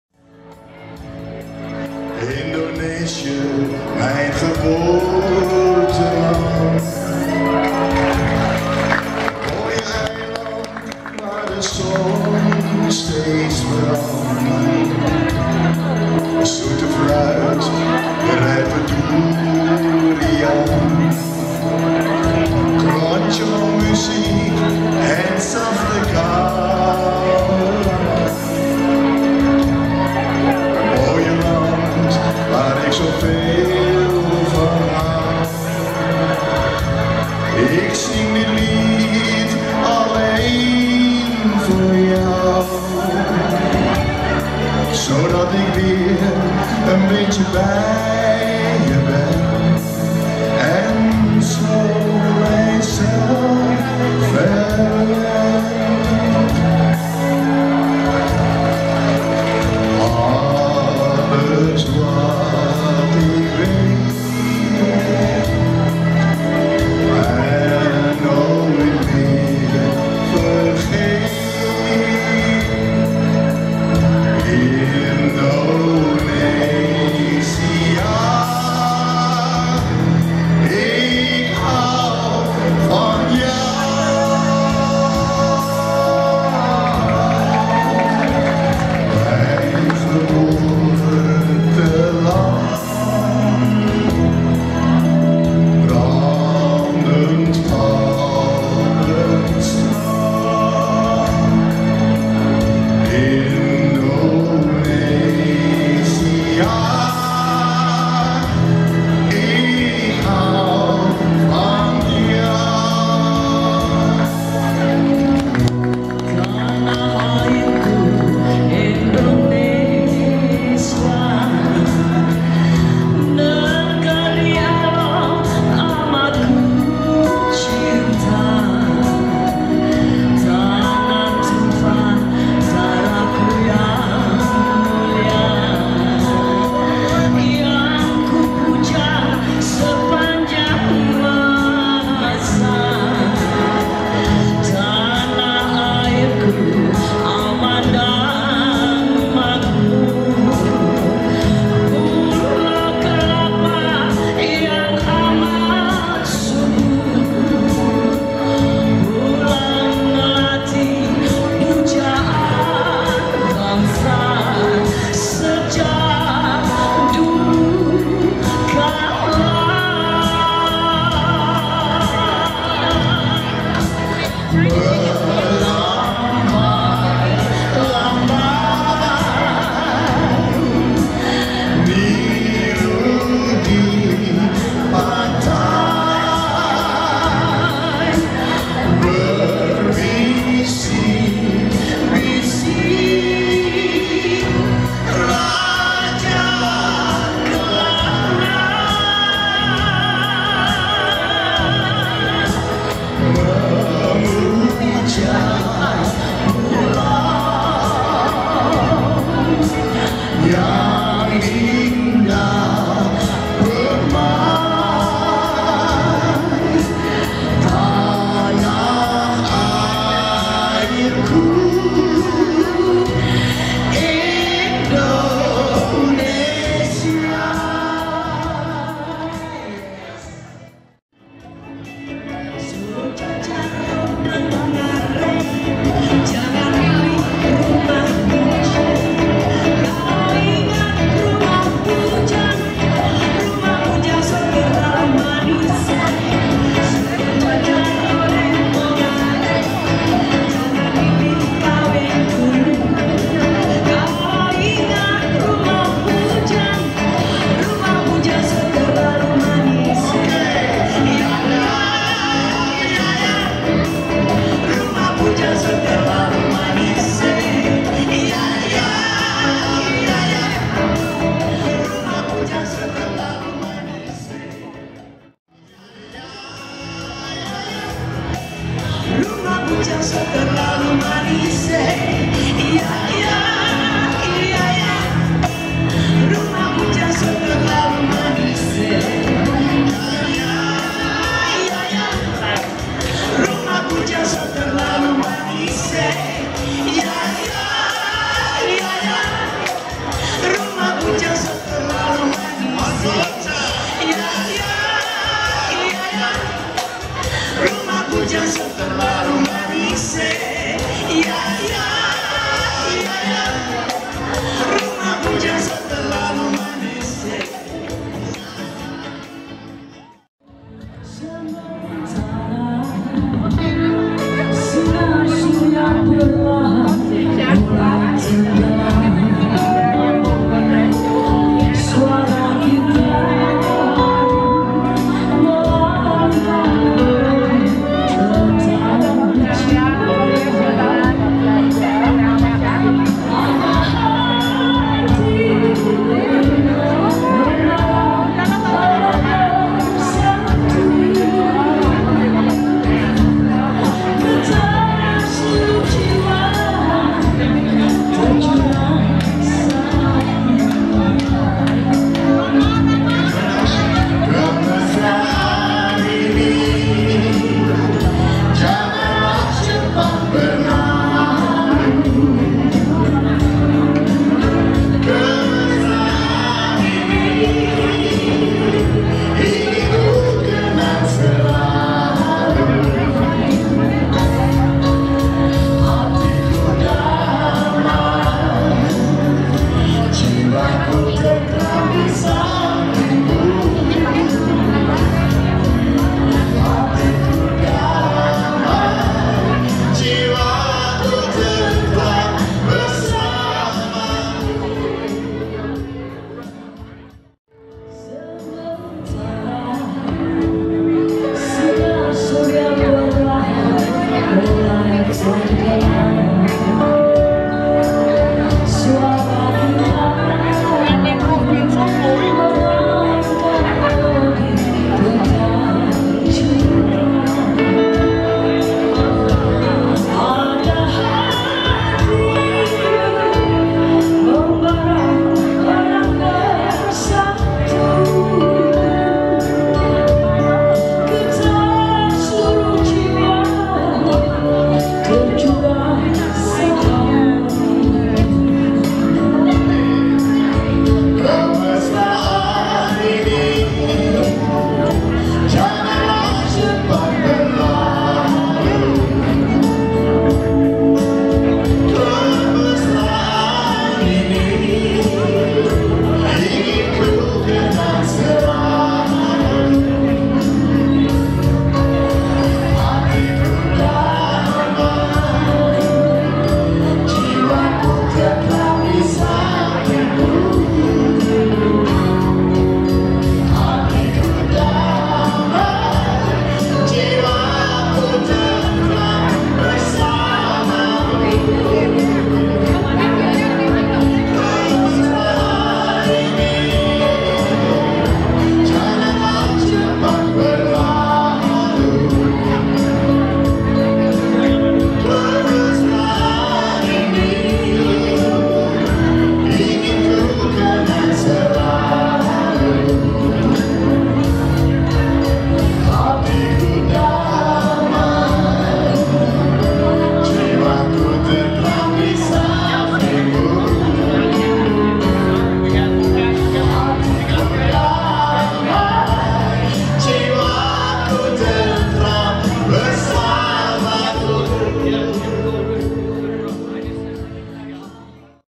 Sawitri Trio Florida Part 2
Sawitri Trio´s performance in Melbourne Florida at The Indonesian Nite Event 2015